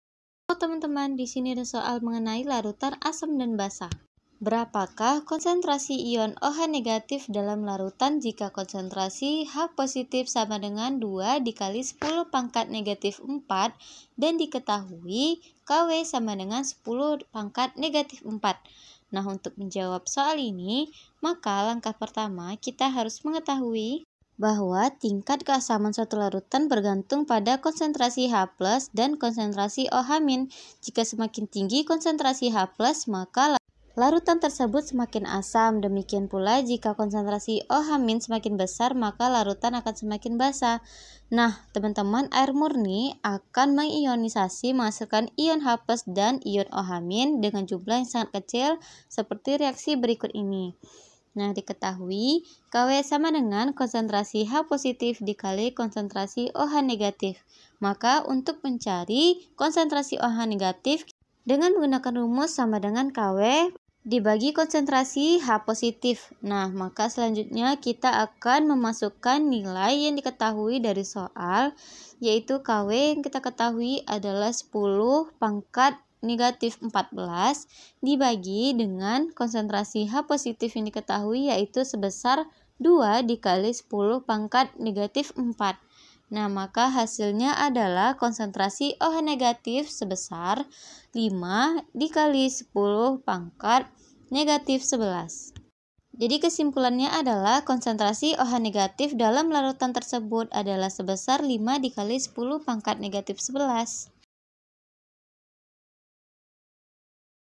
Ion OH⁻ Dalam Larutan Jika Konsentrasi H⁺ = 2 × 10⁻⁴ M | Kimia SMA Kelas XI | Latihan 1
✅ Dalam video ini kita akan membahas:
Berapakah konsentrasi ion OH⁻ dalam larutan jika konsentrasi H⁺ = 2 × 10⁻⁴ M? (Diketahui Kw = 10⁻⁴)

Topik: Larutan Asam Basa
Kelas: 11
Mata Pelajaran: Kimia
Bab: 6
Buku: Aktif Belajar Kimia Untuk SMA/MA Kelas XI